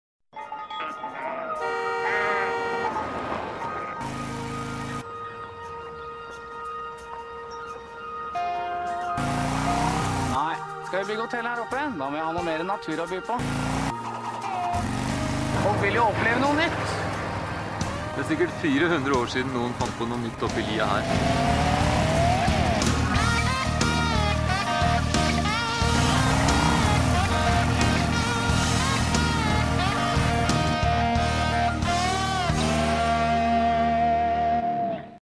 Det er i distriktene det skjer!
SpareBank 1 Alliansen er en suksess! Kanskje takket være denne profilfilmen som ble laget i forbindelse med etableringen av alliansen i 1998?

SpareBank 1 SR-Bank er den største banken i SpareBank 1 Alliansen. De andre store bankene er SpareBank 1 SMN, SpareBank 1 Nord-Norge og Sparebanken 1 Hedmark.